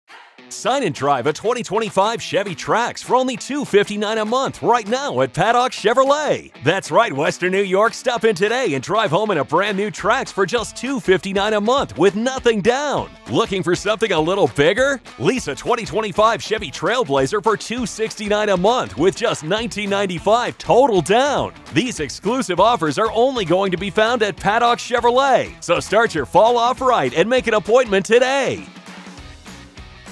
Sign and Drive a 2025 Chevy Trax or Lease a Trailblazer for $269
Sign and Drive a 2025 Chevy Trax for only $259 a month right now at Paddock Chevrolet! That’s right, Western New York, stop in today to drive home in brand new Trax for just $259 a month with nothing down.
Looking for something a little bigger? Lease a 2025 Chevy Trailblazer for $269 a month with just nineteen ninety-five TOTAL down.
These exclusive offers are only going to be found at Paddock Chevrolet so start your Fall off right and make an appointment today!